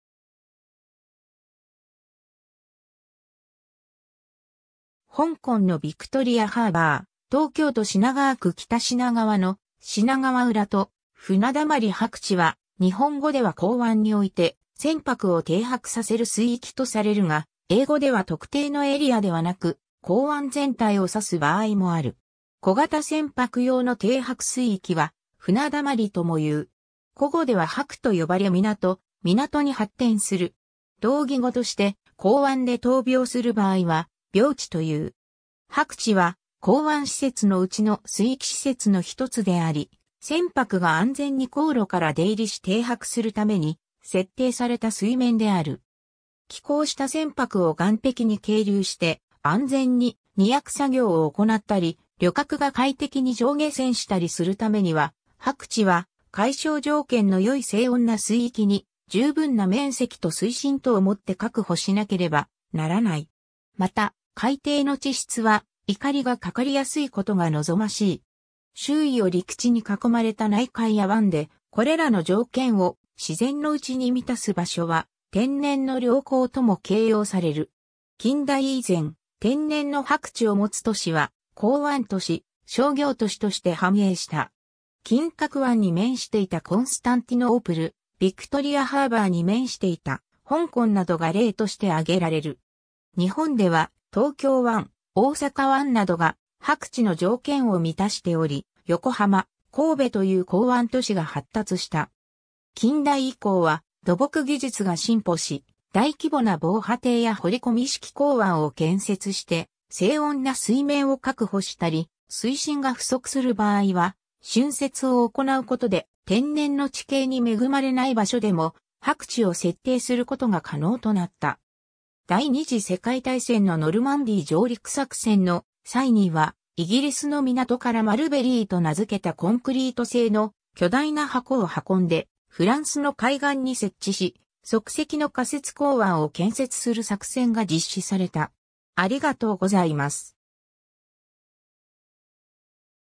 泊地
香港のビクトリア・ハーバー 東京都品川区北品川の「品川浦と船溜まり」 泊地（はくち、英語：harbor/anchorage）は、日本語では港湾において船舶を停泊させる水域とされるが、英語では特定のエリアではなく港湾全体を指す場合もある。
 小型船舶用の停泊水域は船だまり（ふなだまり）ともいう。
古語では泊（とまり）と呼ばれ港・湊に発展する。
同義語として港湾で投錨する場合は錨地（びょうち）という。
 泊地は港湾施設の内の水域施設の一つであり、船舶が安全に航路から出入りし停泊するために設定された水面である。
寄港した船舶を岸壁に係留して、安全に荷役作業を行ったり、旅客が快適に乗下船したりするためには、泊地は海象条件の良い静穏な水域に、十分な面積と水深とを以って確保しなければならない。
また、海底の地質は錨がかかりやすいことが望ましい。
 周囲を陸地に囲まれた内海や湾で、これらの条件を自然のうちに満たす場所は「天然の良港」とも形容される。
近代以前、天然の泊地を持つ都市は港湾都市、商業都市として繁栄した。
金角湾に面していたコンスタンティノープル（イスタンブール）、ビクトリア・ハーバーに面していた香港などが例として上げられる。
日本では東京湾、大阪湾などが泊地の条件を満たしており、横浜、神戸という港湾都市が発達した。
 近代以降は土木技術が進歩し、大規模な防波堤や掘り込み式港湾を建設して静穏な水面を確保したり、水深が不足する場合は浚渫を行うことで、天然の地形に恵まれない場所でも泊地を設定することが可能となった。
 第二次世界大戦のノルマンディー上陸作戦の際には、イギリスの港からマルベリーと名づけたコンクリート製の巨大な箱を運んでフランスの海岸に設置し、即席の仮設港湾を建設する作戦が実施された。